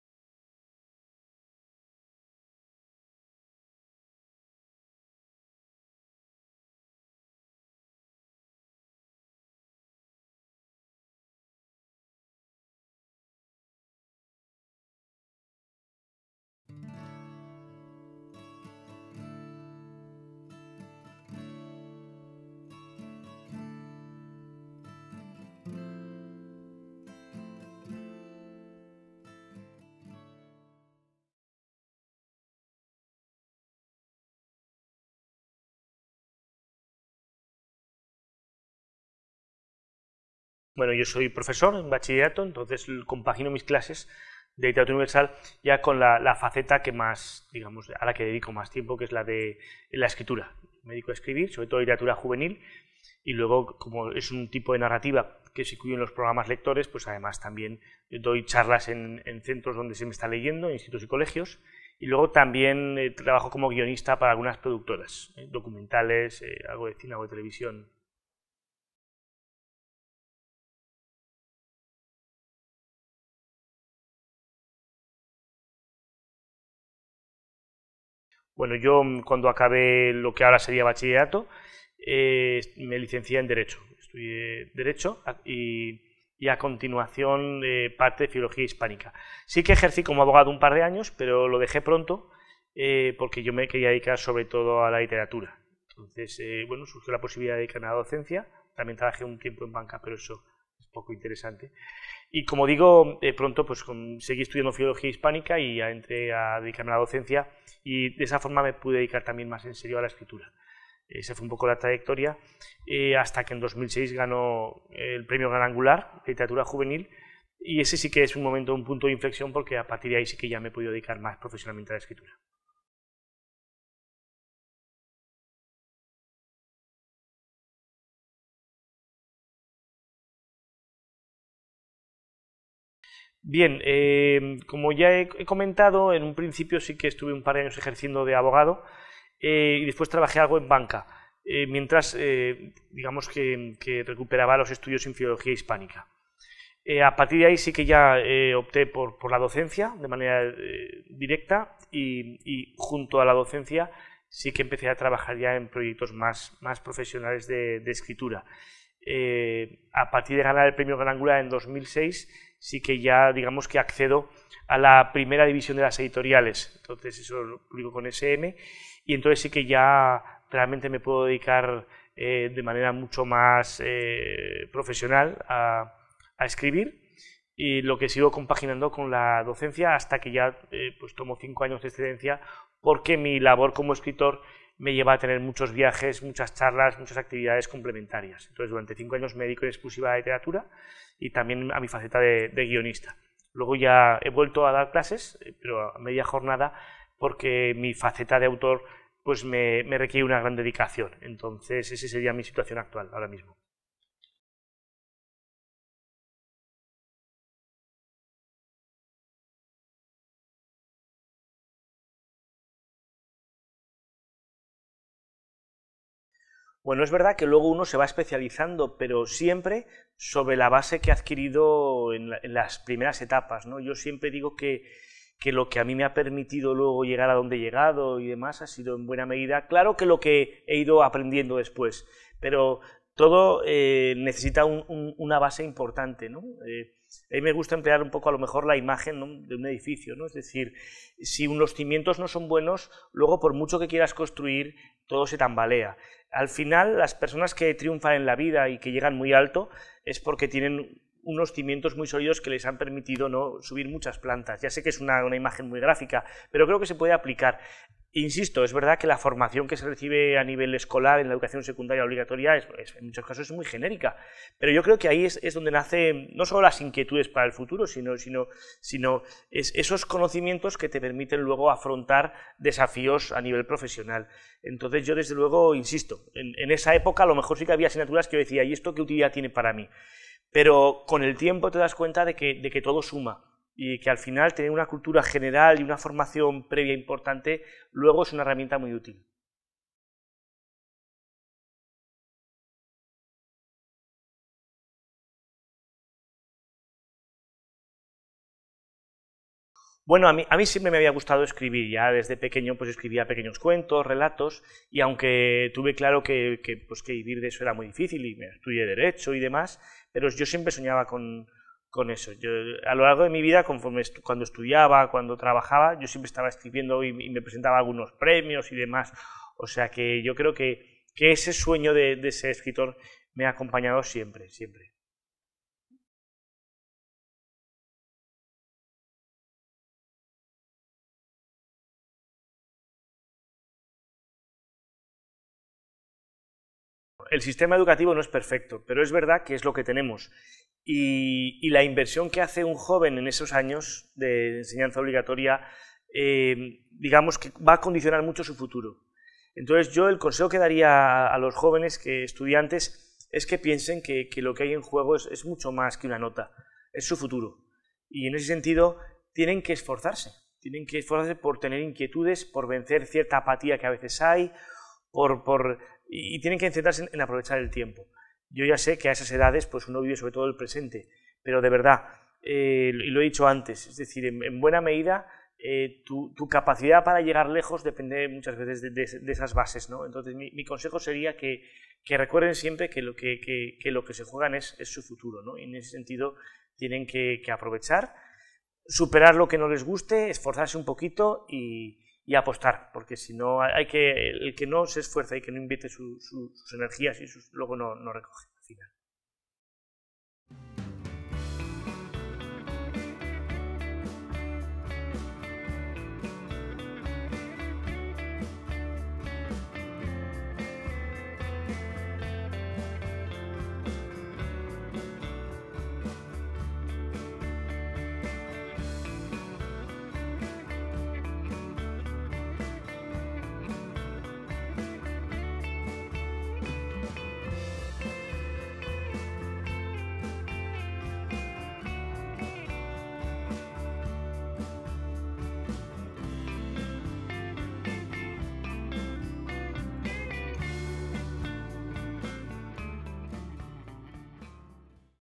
The dream of being a writer - Interview with David Lozano
Although he worked as a lawyer, David always wanted to become a writer.
He tells CREA how studying and training helps you to fulfill your dream.